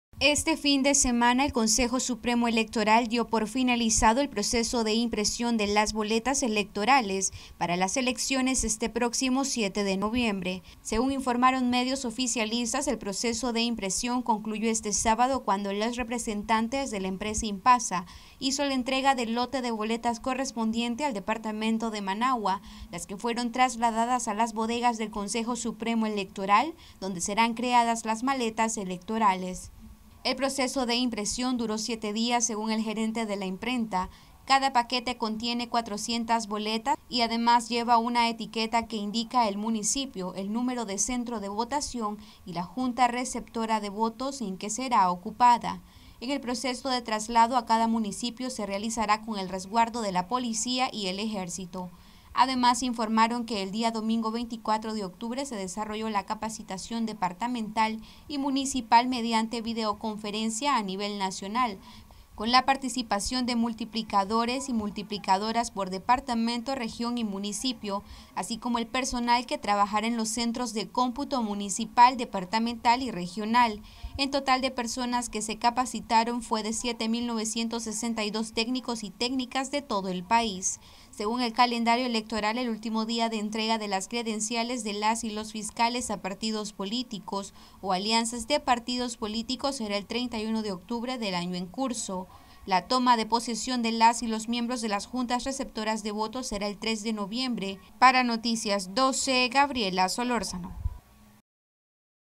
CSE da por concluido proceso de impresión de boletas electorales para elecciones 2021
Noticias12Nicaragua Este fin de semana el Consejo Supremo Electoral dio por finalizado el proceso de impresión de las boletas electorales para las elecciones este próximo 7 de noviembre